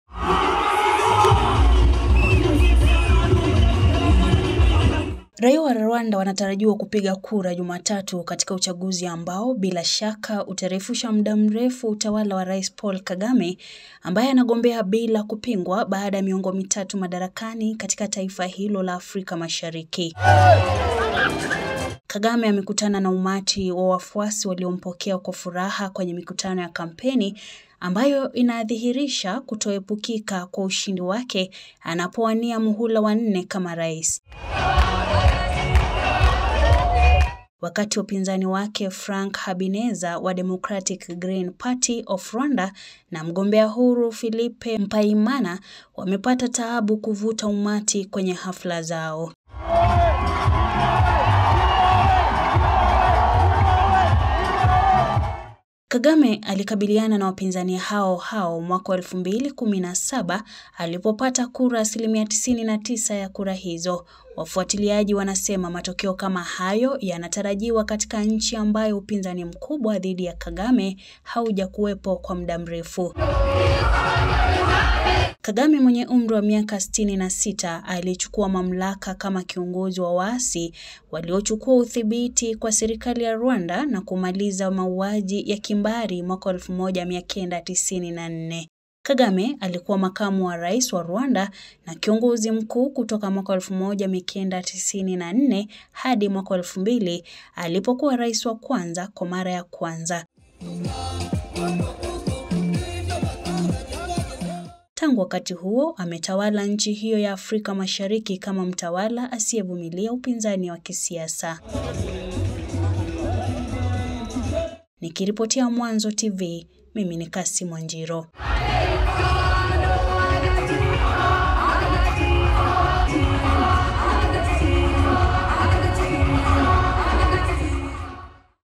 UCHAGUZI RWANDA WAPAMBA MOTO!
Raia wa Rwanda wanapiga kura Jumatatu katika uchaguzi ambao bila shaka utarefusha muda mrefu wa utawala wa Rais Paul Kagame, ambaye anagombea bila kupingwa baada ya miongo mitatu madarakani katika taifa hilo la Afrika Mashariki.